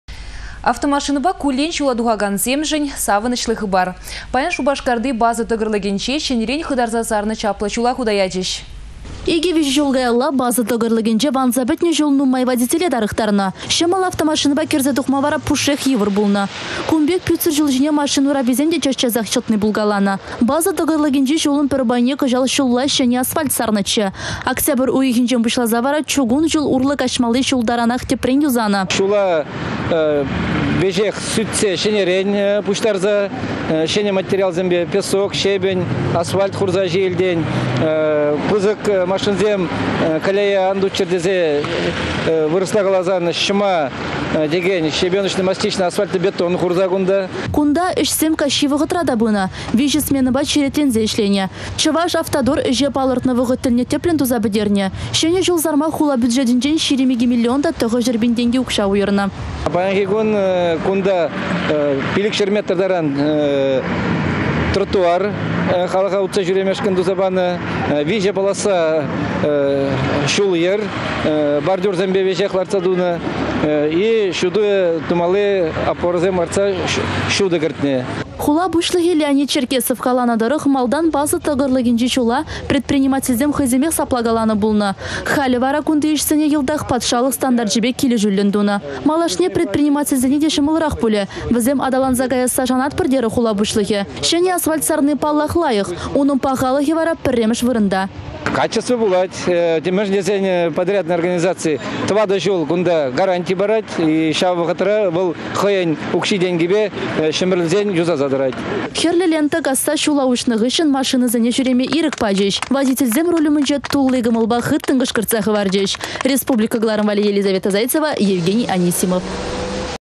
Шупашкарта çĕнĕ çул хута кайрĕ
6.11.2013
Автомашинăпа куллен çула тухакансемшĕн  савăнăçлă хыпар. Паян Шупашкарти База тăкăрлăкĕнче çĕнĕрен хытарса сарнă чаплă çула хута ячĕç.